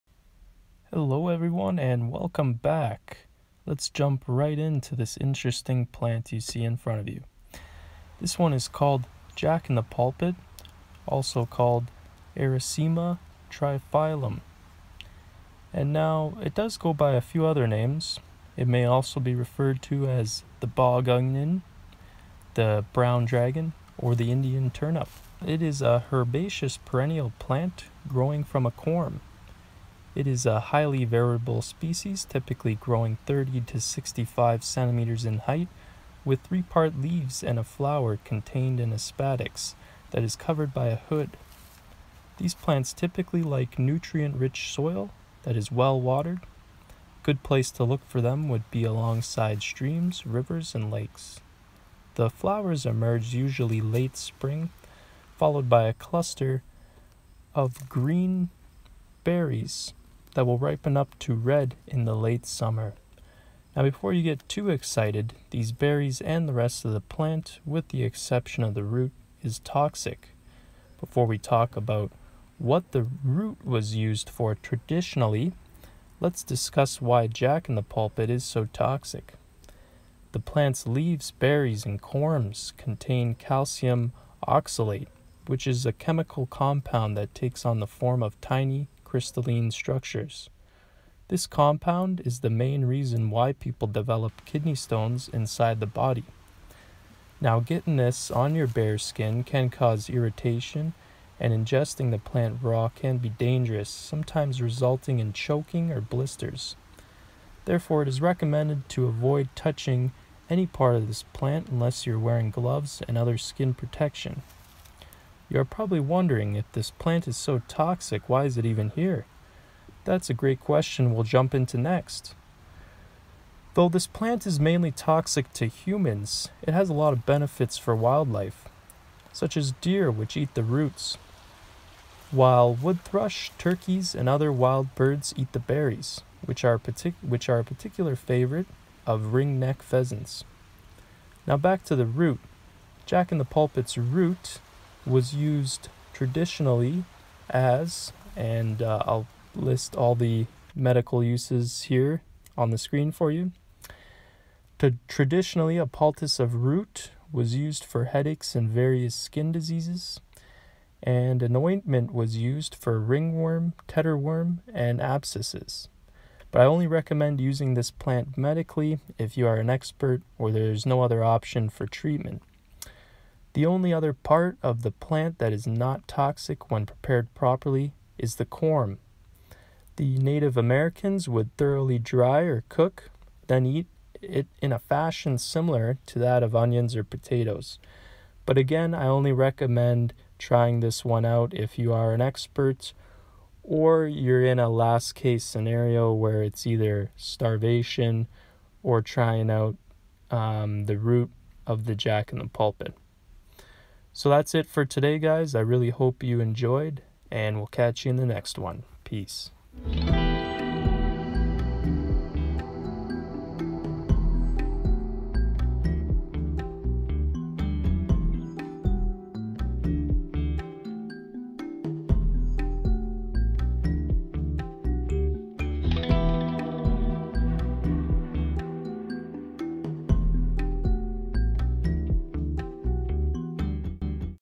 Wild Edibles | Jack in the pulpit
A wild toxic plant that will definitely surprise you with its medical benefits.